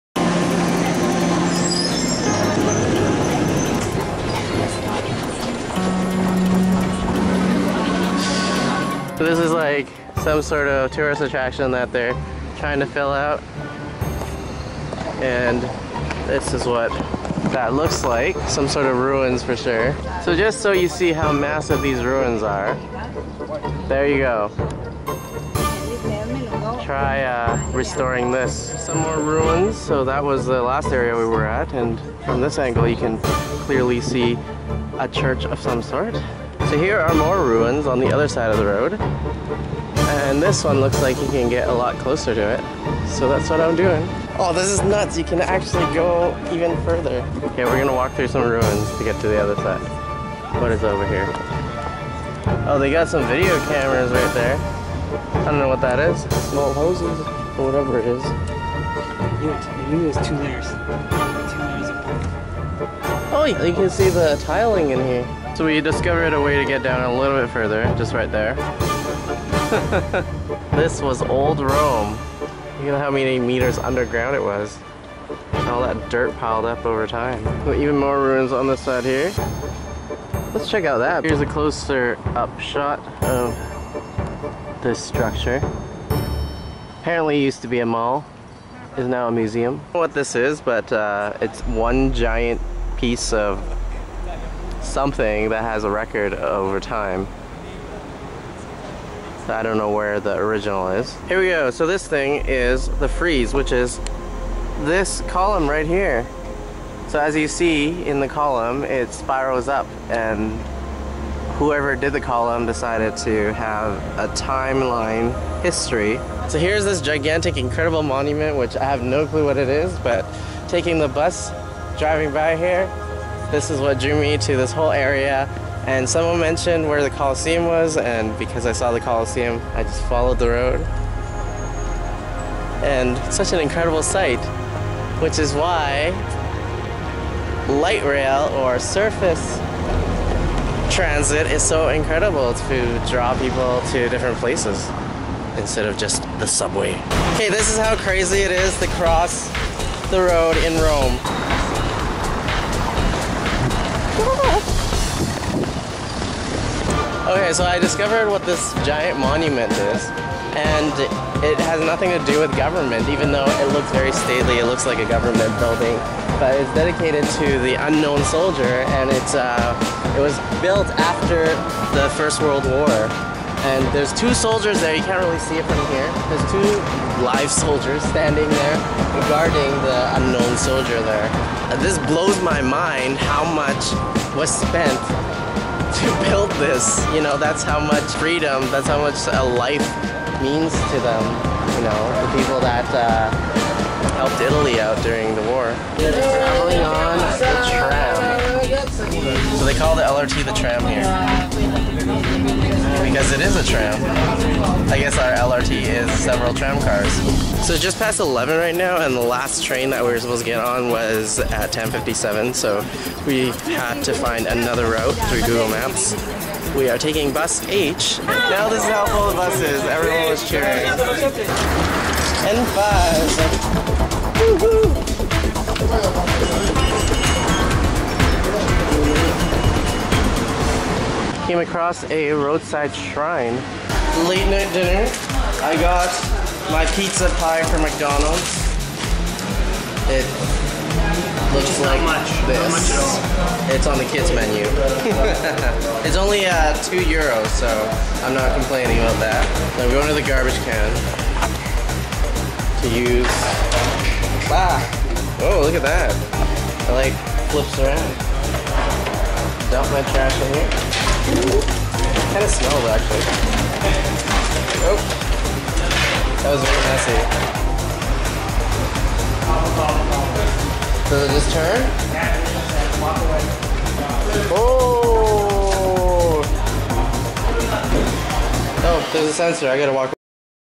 Roman Forum (ROMEing Around 27)
Episode 27 of ROMEing Around, a Millennial Pilgrimage to Italy. Today, we come across the ruins of he Roman Forum, featuring Trajan's Column, as well as Piazza Venezia at night.